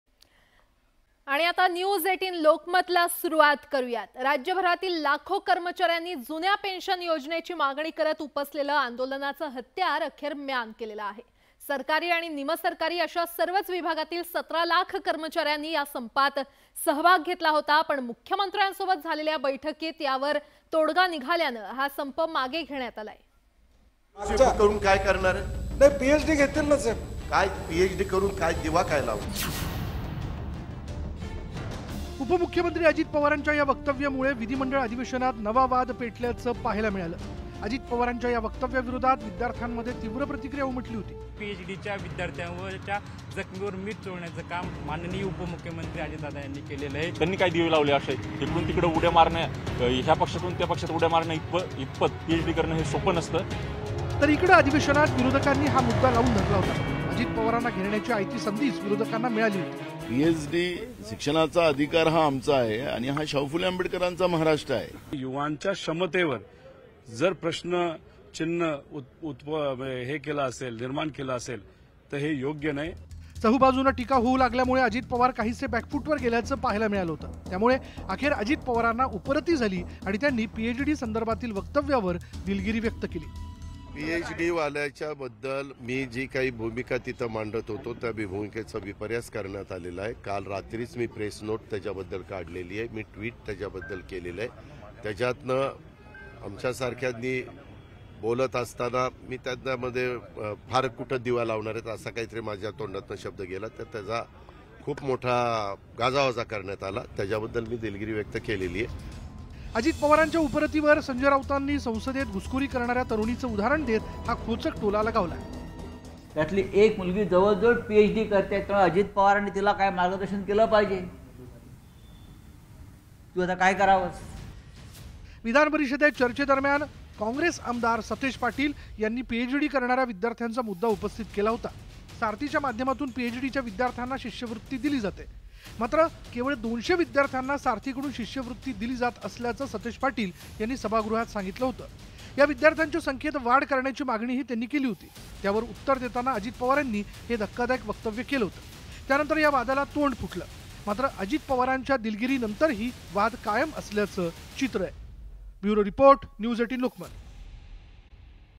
Old Pension Scheme : अजित पवारांची 'पीएचडी'च्या वक्तव्यावरून दिलगिरी | CM Eknath Shinde | Politics
जुनी पेन्शन लागू करा, या मागणीसाठी १४ डिसेंबरपासून संपावर जाण्याचा इशारा राज्य सरकारी कर्मचारी संघटनेच्या वतीने देण्यात आला होता. त्याप्रमाणे आजपासून विविध कर्मचारी संघटना संपावर गेल्या आहेत. या पार्श्वभूमीवर राज्य शासनाने बुधवारी सांयकाळी पाच वाजता मान्यताप्राप्त अधिकारी व कर्मचारी संघटनांच्या नेत्यांना चर्चेसाठी बोलवले होते. बैठकीत मुख्यमंत्री एकनाथ शिंदे, उपमुख्यमंत्री देवेंद्र फडणवीस, उपमुख्यमंत्री व अर्थमंत्री अजित पवार उपस्थित होते. या बैठकीत झालेल्या निर्णयाची माहिती मुख्यमंत्री एकनाथ शिंदे यांनी आज विधानसभेत निवेदनाद्वारे दिली. जुनी पेन्शन योजना लागू करण्यासाठी सरकार सकारात्मक आहे, अशी ग्वाही देताना मुख्यमंत्री शिंदे यांनी सरकारी कर्मचाऱ्यांनी संप मागे घ्यावा, असे आवाहन केले.